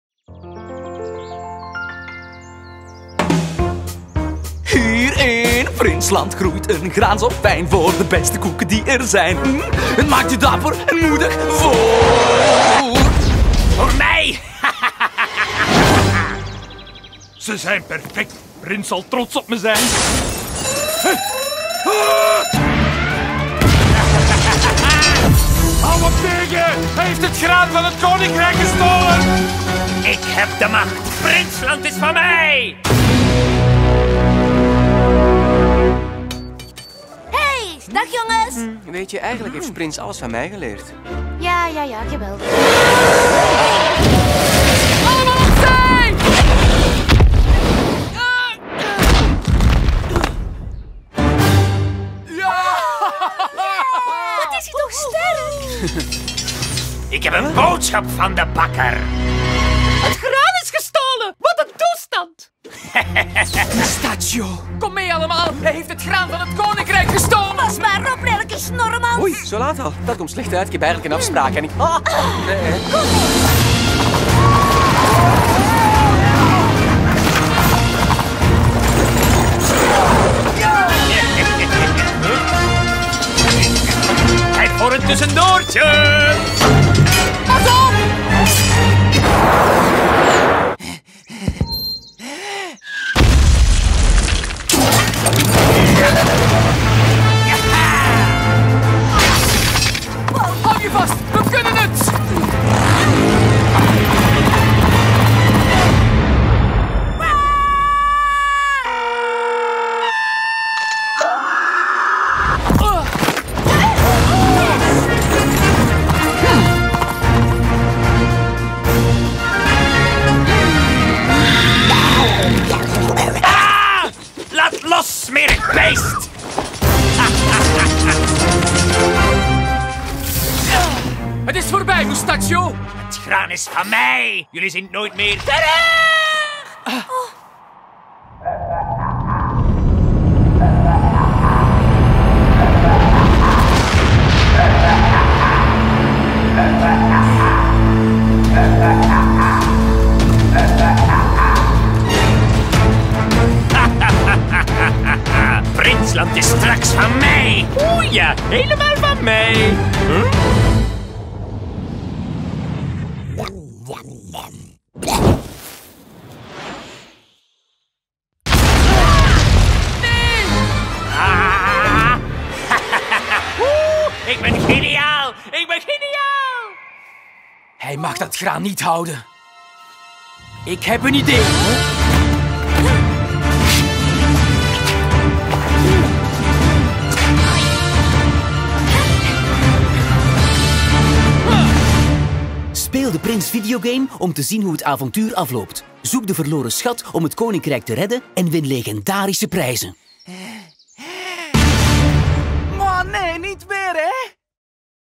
Op zoek naar de verloren schat
De slechte Megalo Moustachio heeft het gouden graan van het koninkrijk gestolen! Laat zien dat jij Princeland kunt redden en maak kans op legendarische prijzen!